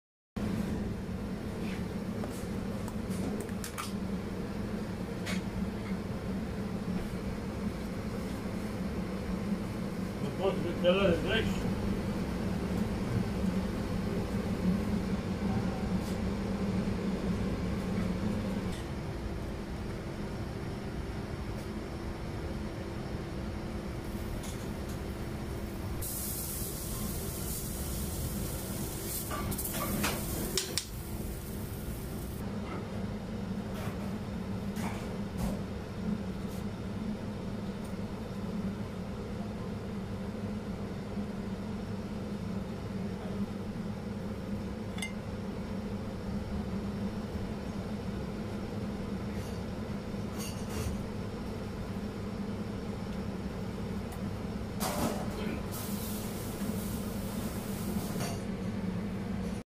Scallops cook | Kammmuschel
A quick video demo showing you how easy it is to cook Scallops at home.